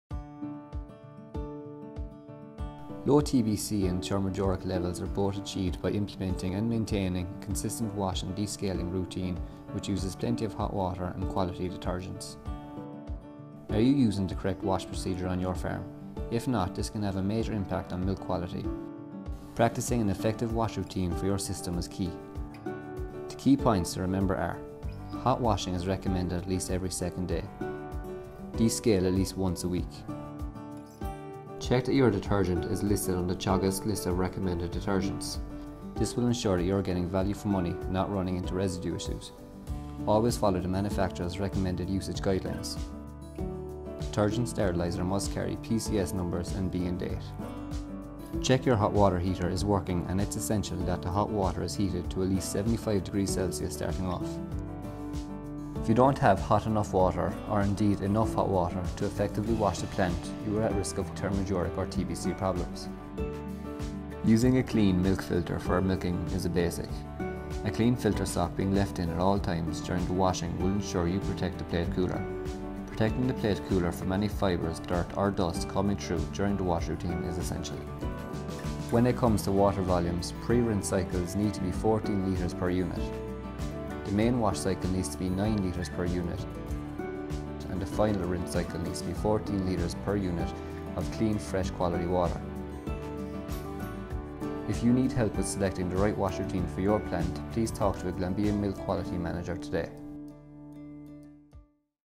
Glanbia Ireland | Choosing the right wash routine for your milking system
A focus on dairy wash routines and teat disinfectants will be useful to ensuring high quality milk production on your farm. Watch our short video that will help you choose the right wash routine for your milking system.